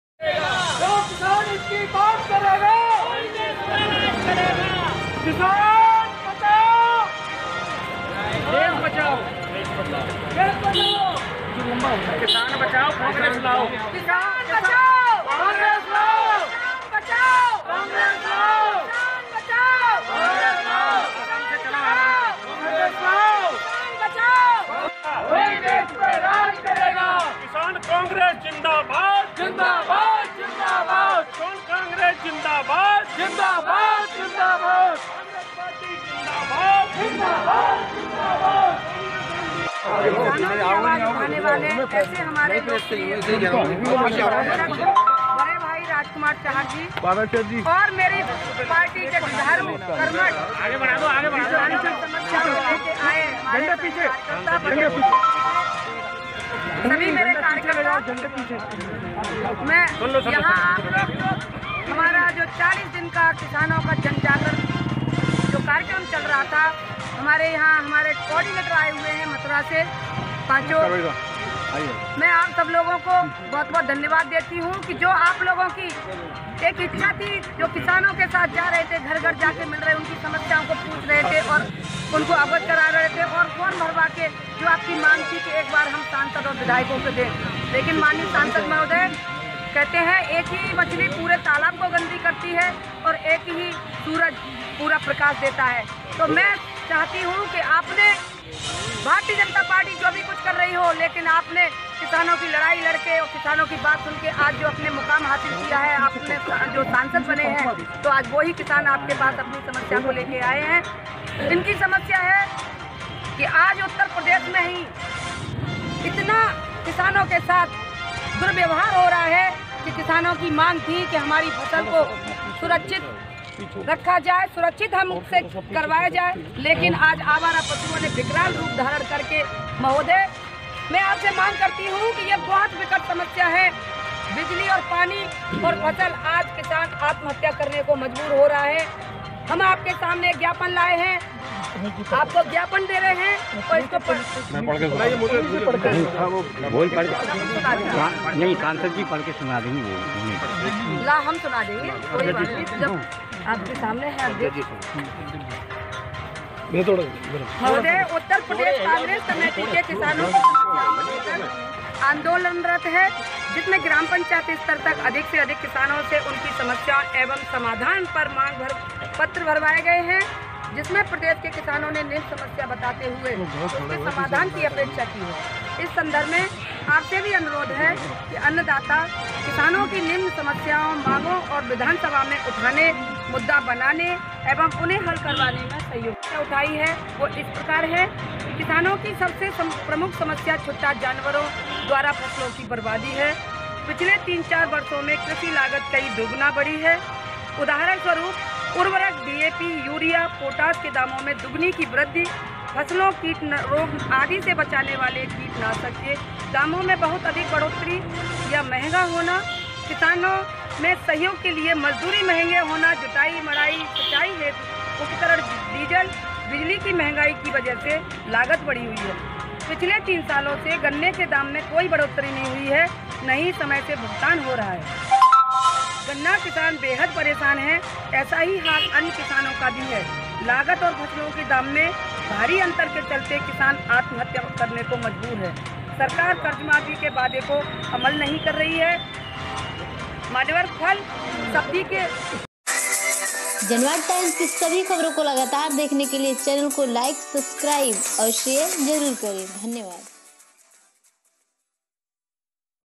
आगरा कांग्रेस कमेटी द्वारा सांसद राजकुमार चाहर को किसानों की समस्याओं के लिए ज्ञापन
Janvad Times : आगरा कांग्रेस कमेटी द्वारा सांसद राजकुमार चहार को किसानों की समस्याओं के लिए ज्ञापन
आज दिनांक 28 फरवरी को राष्ट्रीय महासचिव प्रदेश प्रभारी माननीय प्रियंका गांधी जी और प्रदेश अध्यक्ष माननीय अजय कुमार लल्लू जी के निर्देशानुसार जिला आगरा कांग्रेस कमेटी द्वारा फतेहपुर सीकरी लोकसभा सांसद आदरणीय राजकुमार चहार को किसानों की समस्याओं के लिए ज्ञापन दिया गया ज्ञापन को संबोधित करते हुए श्रीमती मनोज दीक्षित ने कहा कि विगत सालों में किसान की अवस्था बद से बदतर हालत हो गई है मोदी और योगी दोनों ही किसान विरोधी हैं किसान आत्महत्या कर रहा है किसानों की सबसे प्रमुख समस्या छुट्टा जानवरों द्वारा फसल की बर्बादी है कस लागत कई गुना बढ़ गई है किसान को फसलों का उचित मूल्य नहीं मिल रहा हैऔर आपदाओं के चलते किसानों की फसलें तबाह हो जाती हैं लेकिन फसल बीमा के नाम पर बहुत ही कम किसानों को लाभ मिल पा रहा है जबकि बीमा कंपनियां मालामाल होती जा रही हैं जिला अध्यक्ष श्रीमती मनोज दीक्षित ने कहा कि माननीय प्रियंका गांधी जी और माननीय अजय कुमार लल्लू किसानों की समस्याओं को लेकर चिंतित हैं और आंदोलनरत हैं किसान जन जागरण अभियान के तहत ग्राम पंचायत स्तर तक किसानों से उनकी समस्याओं एवं समाधान पर मांग पत्र भरवाए गए हैं
ज्ञापन लेने माननीय सांसद राजकुमार जहाजी स्वयं आए सततम् उन्होंने जिला अध्यक्ष श्रीमती मनोज दीक्षित का मिठाई खिला कर स्वागत किया और आश्वासन दिया की मैं किसान का बच्चा हूं बेटा हूं किसानों की समस्याओं से भली-भांति परिचित हूं तथा हर संभव किसानों की समस्याओं को समाधान करने के लिए तत्पर रहूंगा
ज्ञापन देने वालों में सर्वश्री चंद्र मोहन पाराशर सर्वोदय अनुवाद सिद्दीकी रामदत्त दिवाकर विष्णु शर्मा ओमप्रकाश सिकरवार नरेश सिकरवार नूतन बिहारी पारीक रामसेवक वर्मा मोहन चौधरी राजकुमारी चौधरी विनेश सेलवॉल राजेश पुरी पंकज जैन सायरा बानो अतुल यादव जगदीश लवानिया हरिओम दीक्षित राजा खानम सुशील वर्मा दयाराम प्रजापति गोरेलाल शर्मा टीकाराम शर्मा भीकाराम शर्मा नगीना रुखसाना सारा बानो शिवा आदि लोग उपस्थित रहे